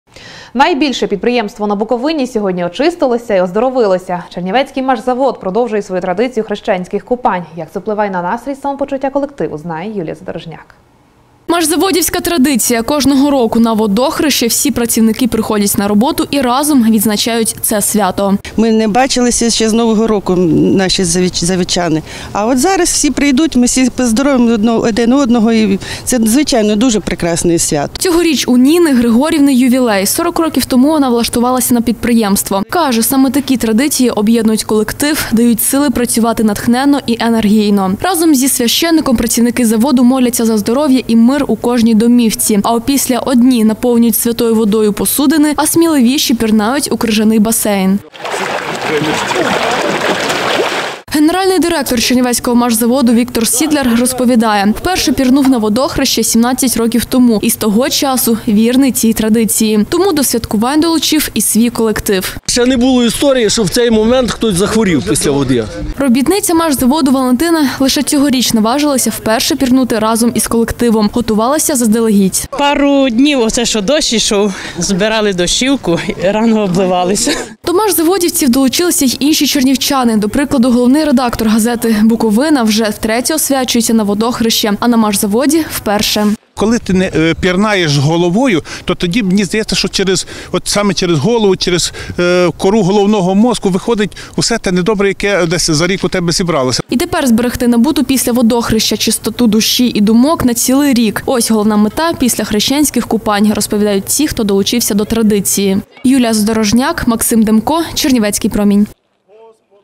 Так загартовується «Машзавод»
160119 Машзаводівці продовжують традицію хрещенських купань